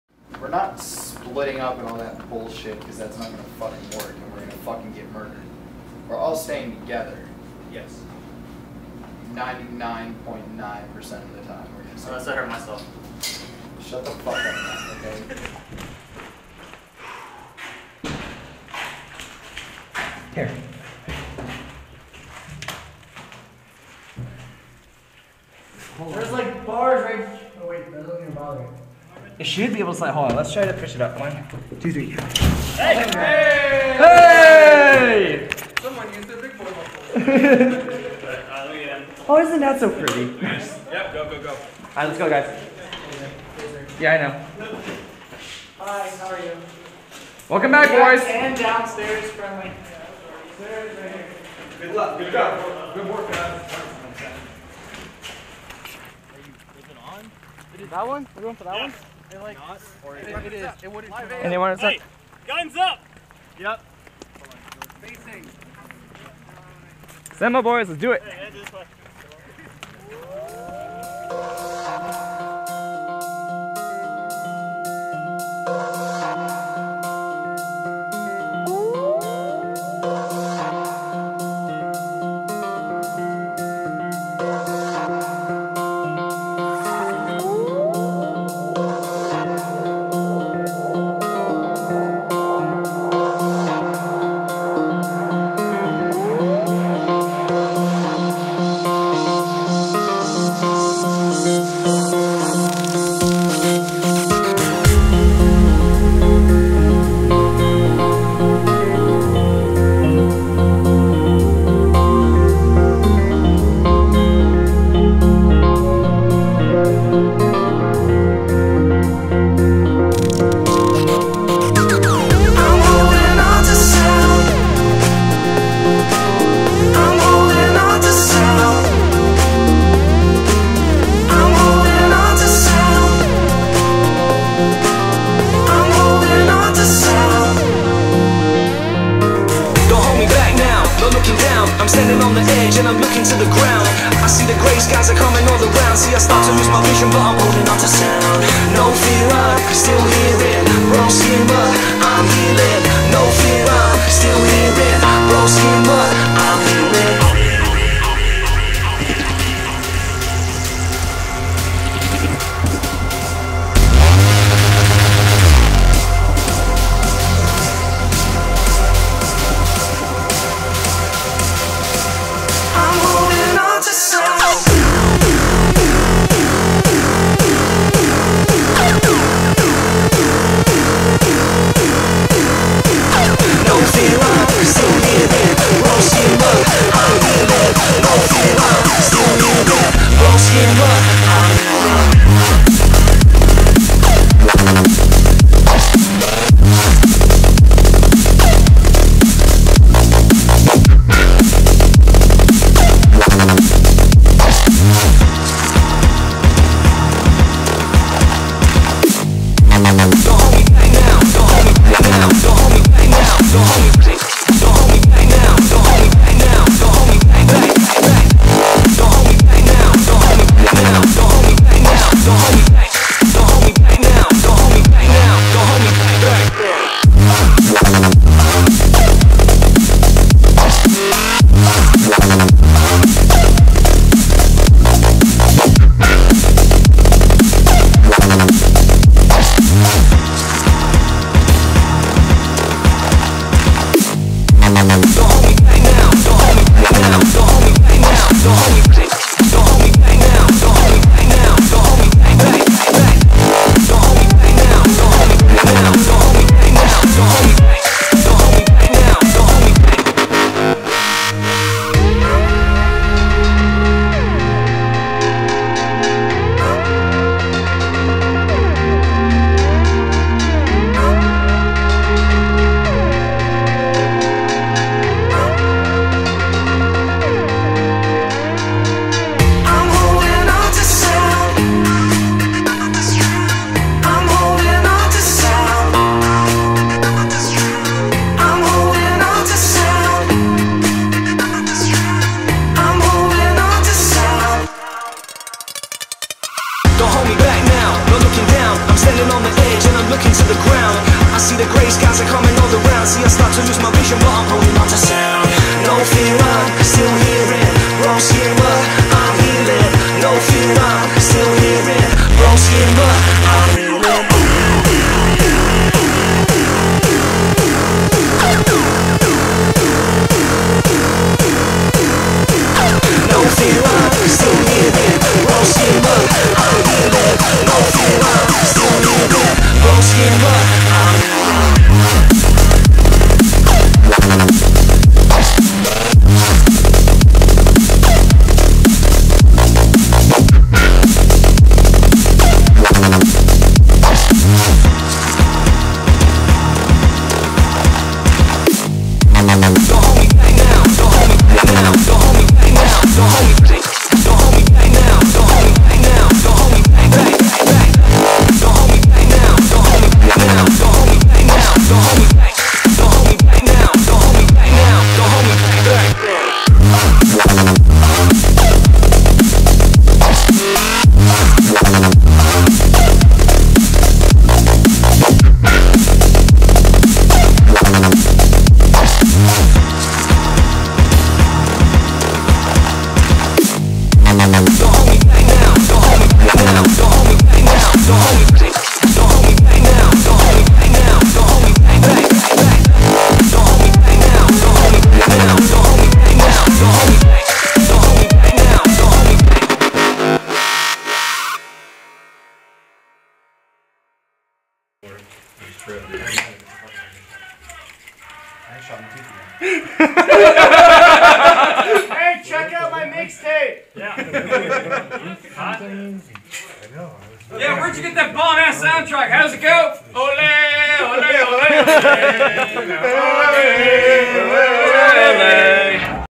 OP:Stonewall Montage 2016 -What a Blast-
"I'll never do a milsim event" said Hi-C once some years ago.  Yeah, ok buddy.

So I compiled my clips and stuffed them into a 6 minute montage.  Thanks again to MSATO and Centurion Airsoft for a great time and what an amazing AO.

I do have a first impressions and final thoughts video coming out next week that I recorded after Stonewall but I wanted to edit it because its a whopping 29 minute video.  I thought I talked like 15 minutes, no its 29.

LASTLY, check out my facebook page for photos from the event!  I got a couple and other photos from other games I play at the local PEVs field.